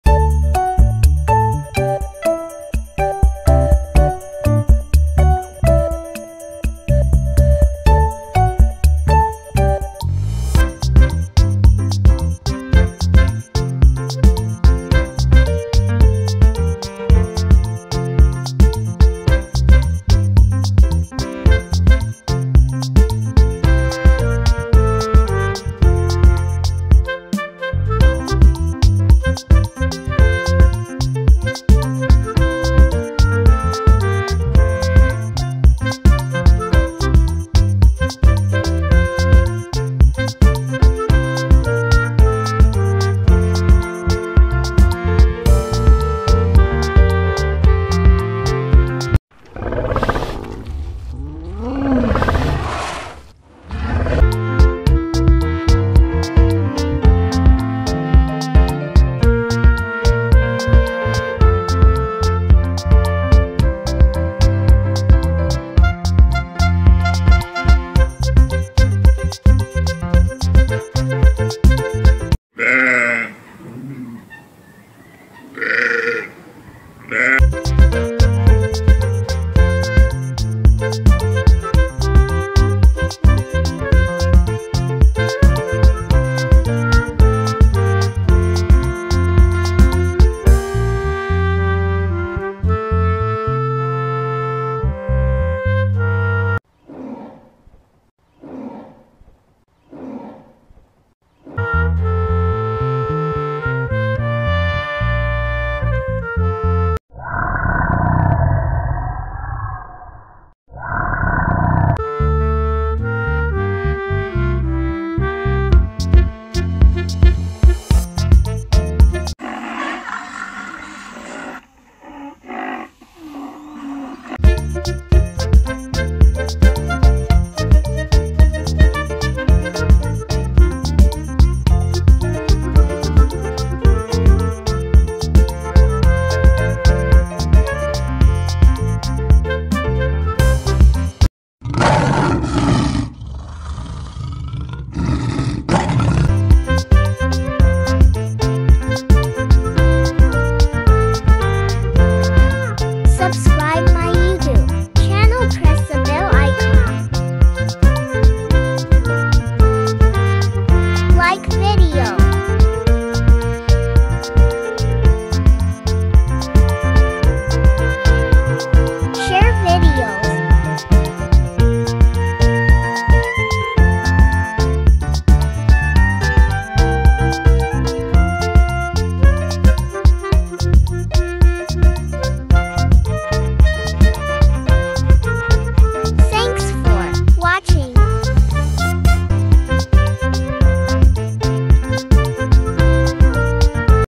Two World Largest Dinosaur animal and One Farm animal Sheep Three Wild animals Tiger Rhino lioness
How are you friends I hope you are fine
Today our video is very interesting
let's start our video
Social media link.